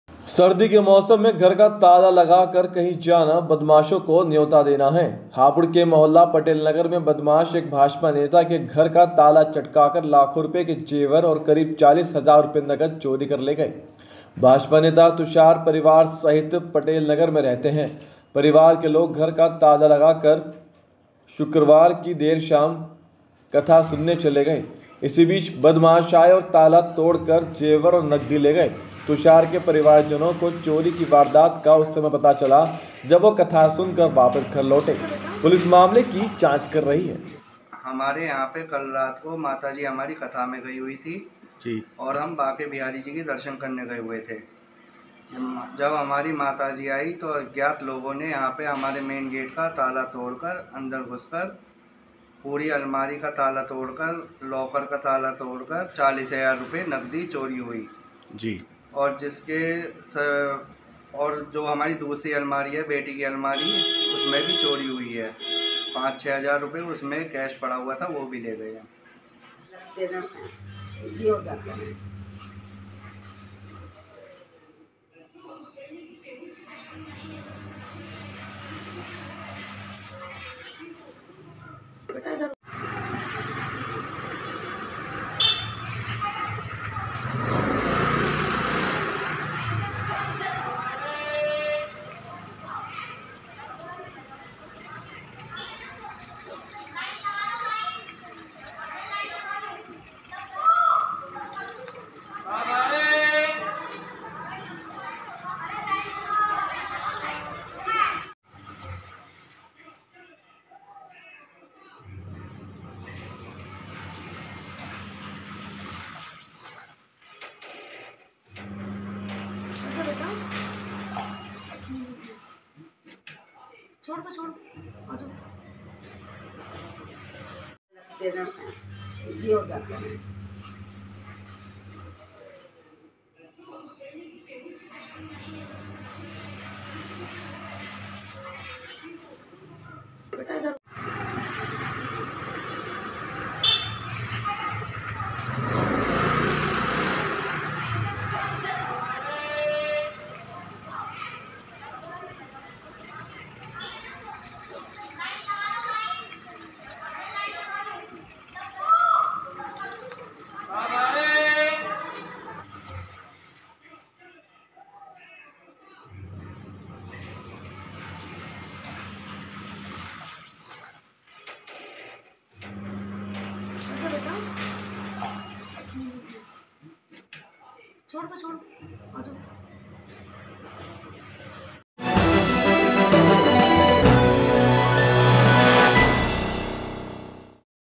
घर का ताला लगाकर जाना बदमाशों को न्यौता देना
हापुड़, सीमन (पंजाब केसरी) :  सर्दी के मौसम में घर का ताला लगा कर कहीं जाना, बदमाशों को न्यौता देना है। हापुड़ के मौहल्ला पटेल नगर में बदमाश एक भाजपा नेता के घर का ताला चटका कर लाखों के जेवर व करीब 40 हजार रुपए नकद चोरी कर ले गए।
भाजपा नेता तुषार परिवार सहित पटेल नगर में रहते है। परिवार के लोग घर का ताला लगा कर कथा सुनने चले गए। इसी बीच बदमाश आए और ताला तोड़कर जेवर व नकदी ले गए। तुषार के परिवारजनों को चोरी की वारदात का उस समय पता चला जब वे कथा सुनकर घर वापिस लौटे। पुलिस जांच कर रही है।